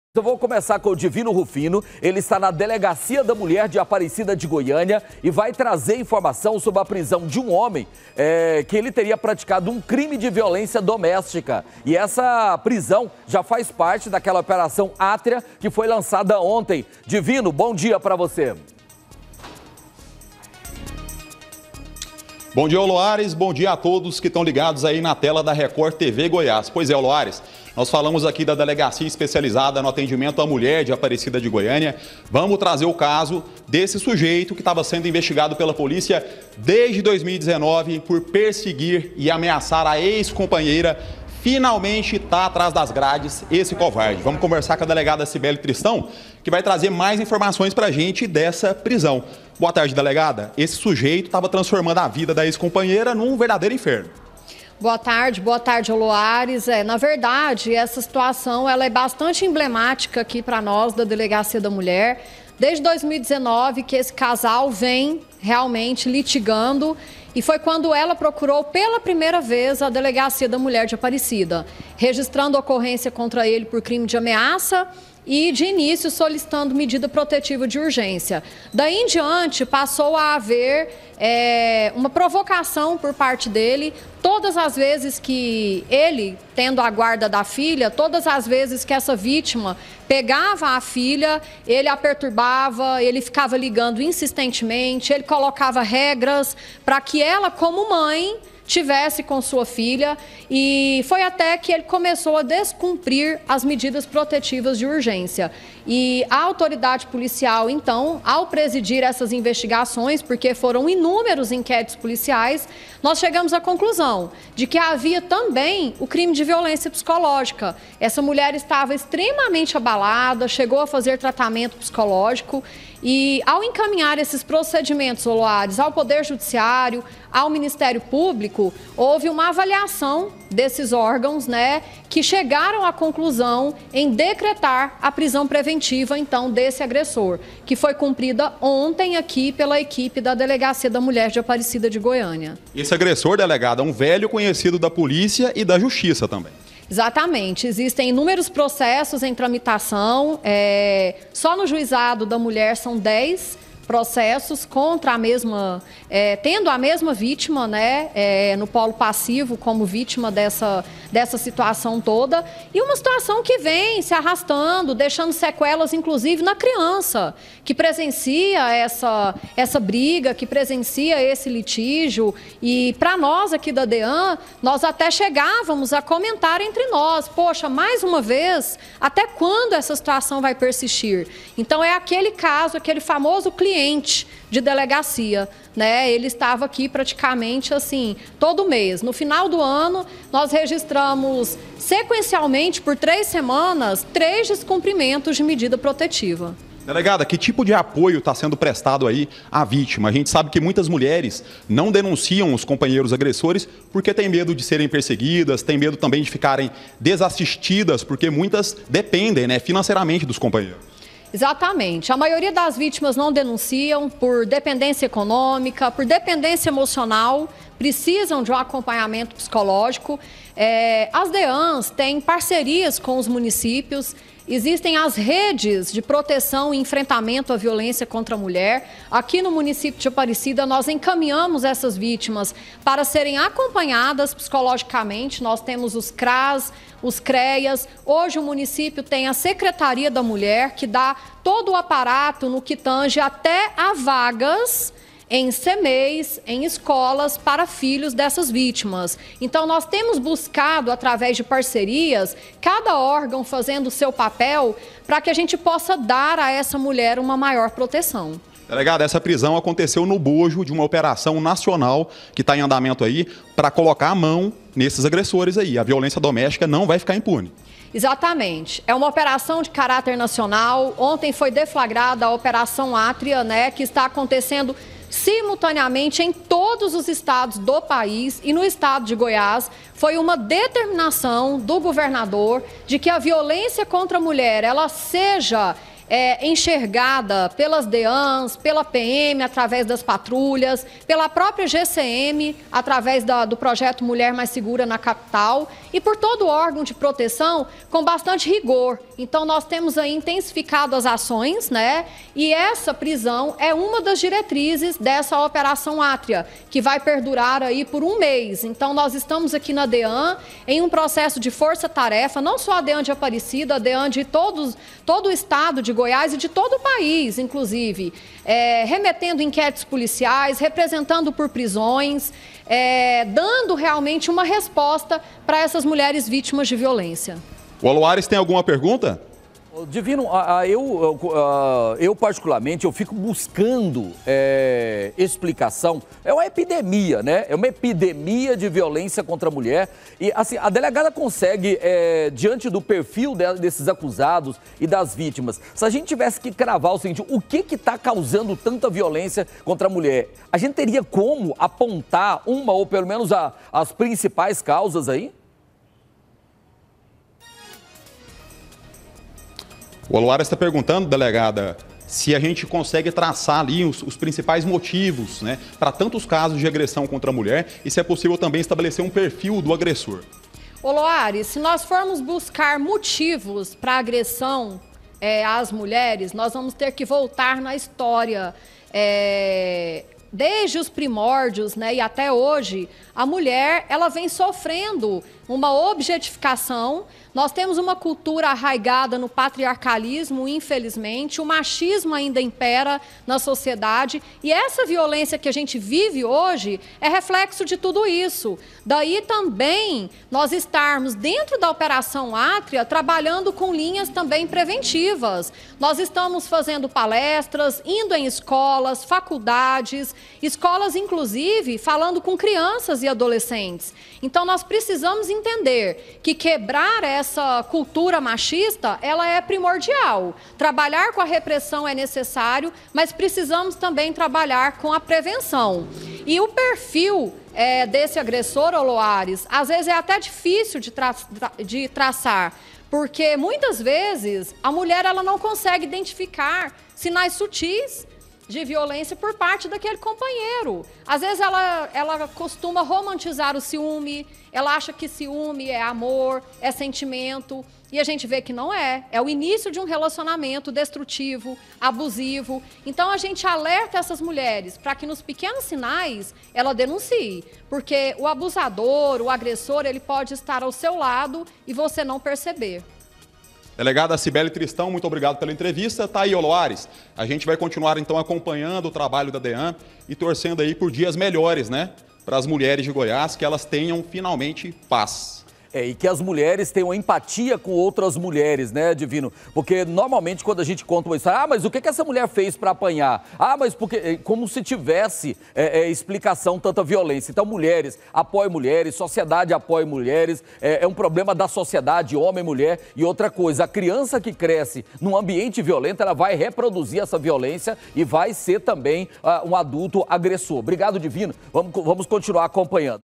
AGRESSOR PRESO: MULHER ERA VÍTIMA DE VIOLÊNCIA E PERSEGUIÇÃO
DIVINO RUFINO ESTÁ NA DELEGACIA DA MULHER DE APARECIDA TRAZ INFORMAÇÕES SOBRE A PRISÃO DE UM HOMEM POR CRIME DE VIOLÊNCIA DOMÉSTICA./ É A PRISÃO FAZ PARTE DA OPERAÇÃO ÁTRIA LANÇADA ONTEM.